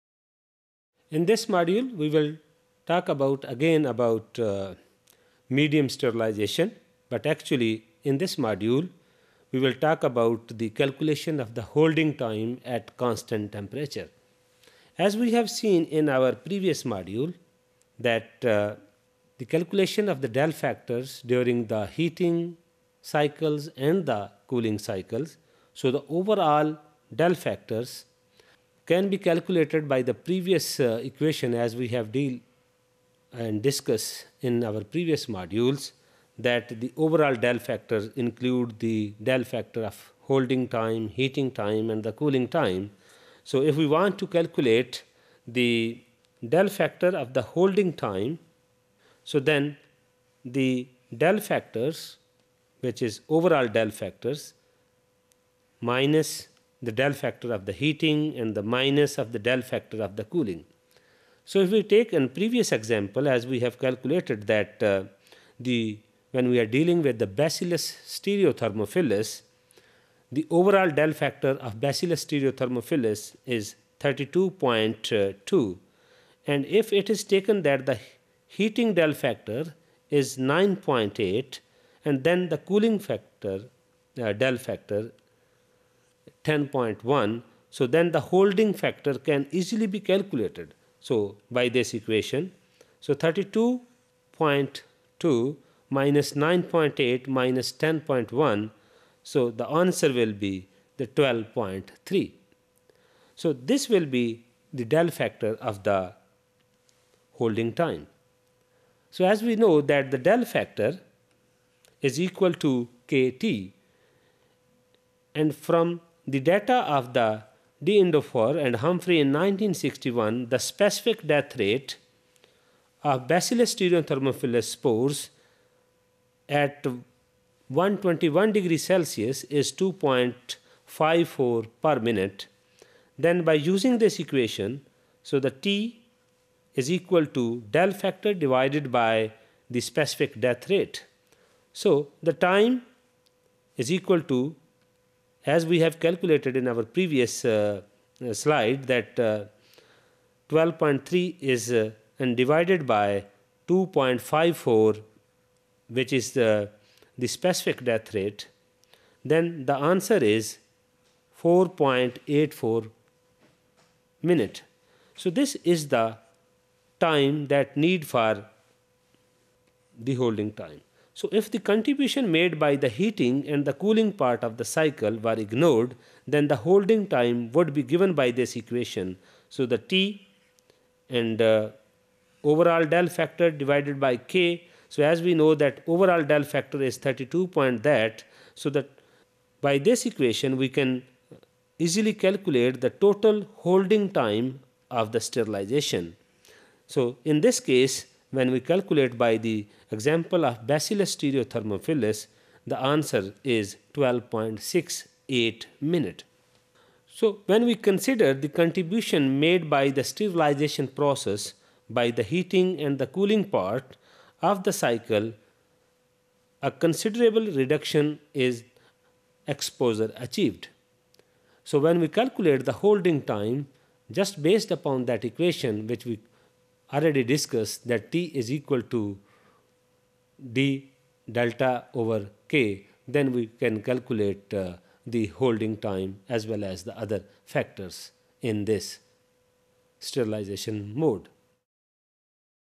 Calculation Holding time at Constant temp | Principles of Biochemical Engineering | BT513T_Topic069
Topic069: Medium Sterilization: Calculation of Holding time at Constant temp